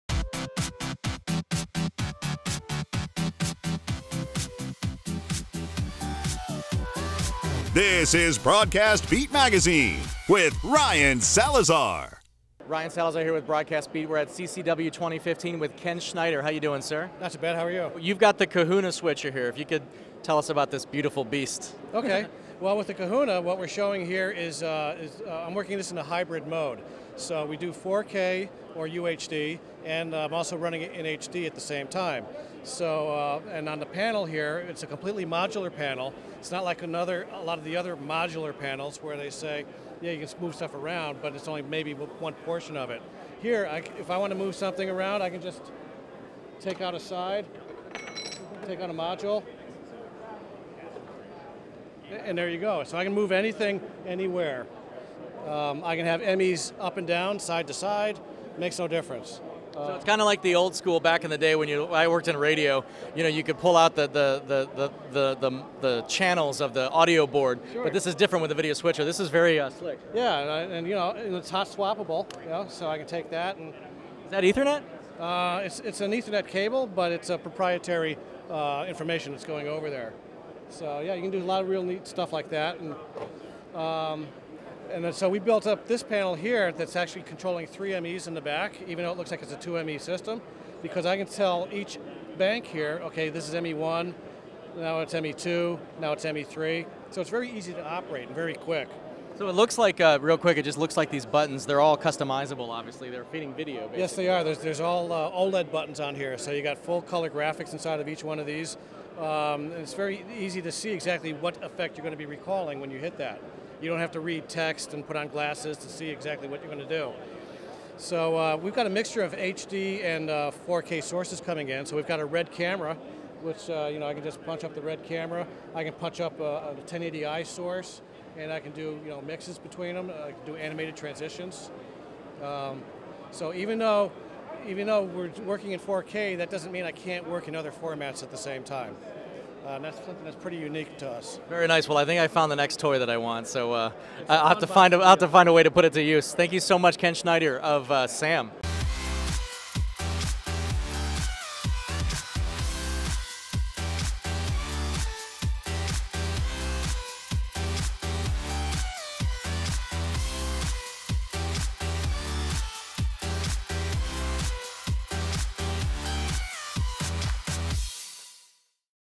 The Kahuna Switcher from Snell Advanced Media (SAM) at CCW+SATCON
Ken Schneider of Snell Advanced Media talks about the Kahuna Switcher at the 2015 CCW+SATCON Show in New York City.

The most powerful production switcher
The ‘Daddy’ of the Kahuna range and one that packs a punch for any media organization that demands the best in quality, creativity, innovation, flexibility and above all reliability. Be it sports, music, news, live events or multiple site application the Kahuna 9600 can be configured exactly the way you need it to produce the best results without any compromise.

Kahuna 9600 is available today in SD, HD, 1080p, 4K and IP. It benefits from Make M/E enabling you to maximize your resources and allocating where you see fit and is completely interchangeable depending on your production. Teamed up with Kahuna Maverik, SAM’s modular panel, Kahuna 9600 is truly the most powerful and adaptable production switcher that liberates any media organization in their creativity when designing their production suites, capitalizing on future cost savings.

With such an extensive feature set and advanced technology Kahuna 9600 will always reward you with the highest caliber productions and peace of mind that it just works.

Switcher for complex environments
Suited for larger scale 4K, 1080p and now IP environments, Kahuna 9600 supports the most complex productions including multiple studio operations effortlessly. Combining the advanced 3D DVE, 2.5D DVE resize engines and easy macro building the Kahuna 9600 offers operators a large suite of creativity at their fingertips.

Match this with the innovative modular panel Kahuna Maverik and you’ve got yourself the most powerful production switcher system in the industry.